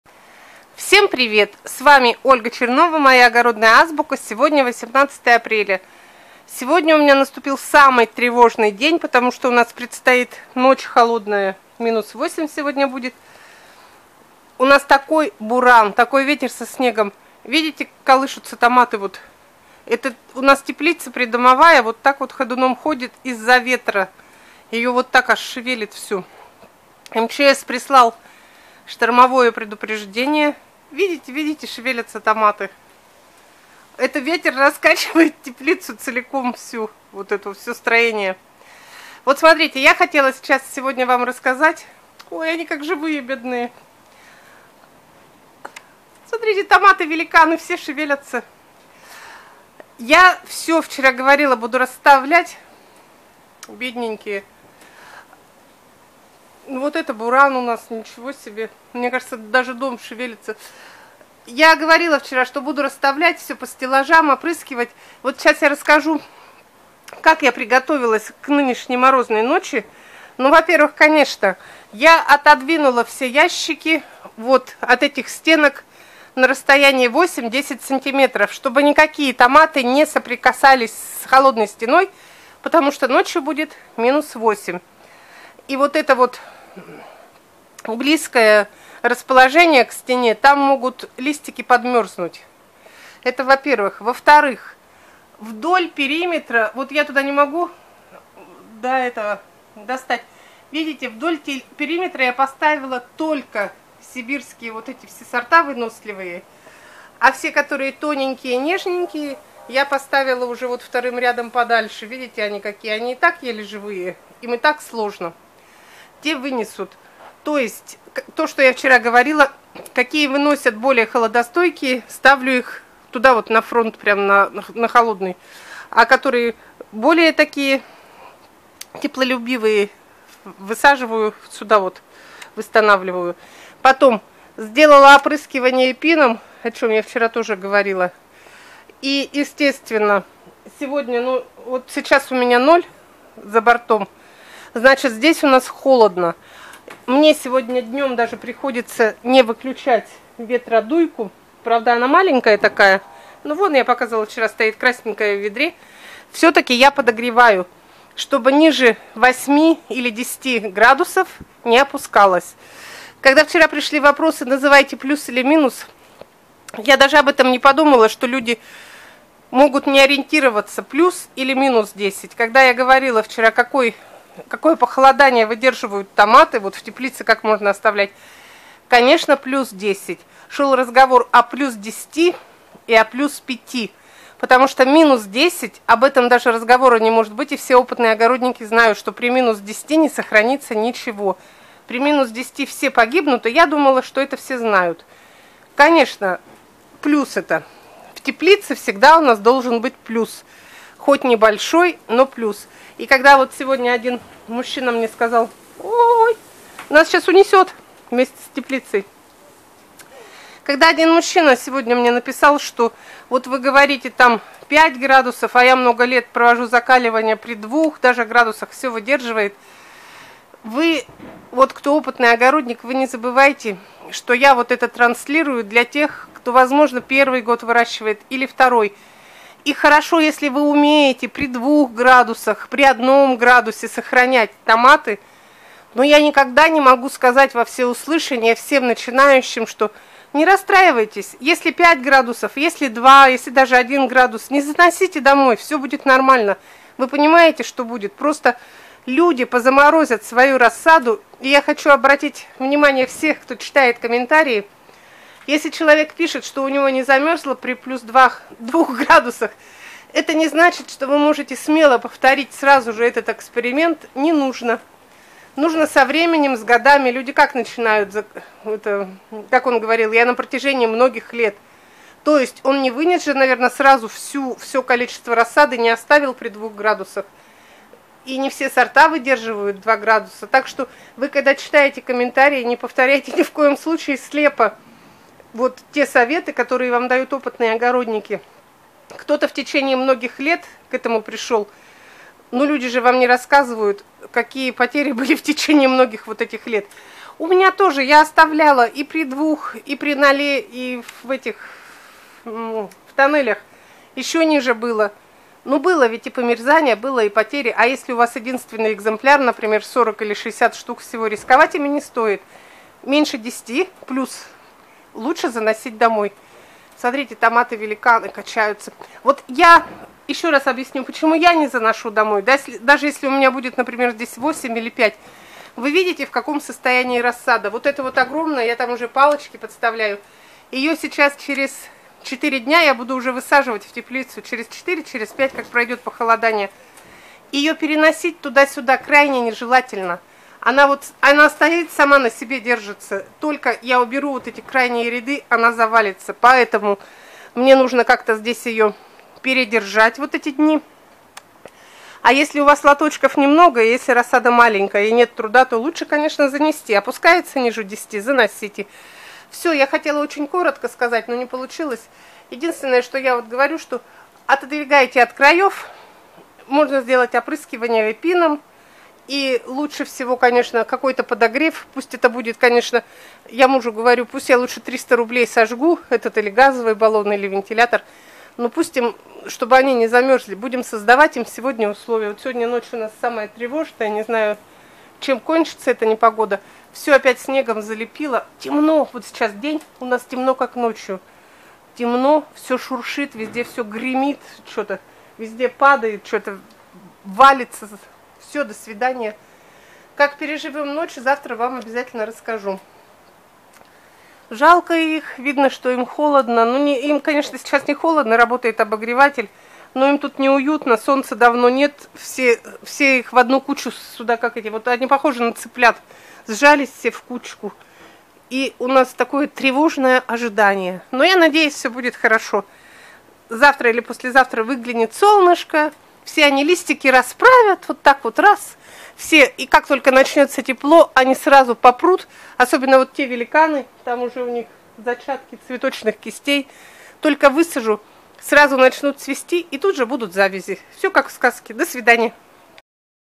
К МОРОЗУ ПОДГОТОВИЛИСЬ! Ольга Чернова.
У нас ночью ожидается понижение температуры до -8 градусов. В этом видео я рассказываю, как я подготовила свою теплицу с рассадой к такому морозу.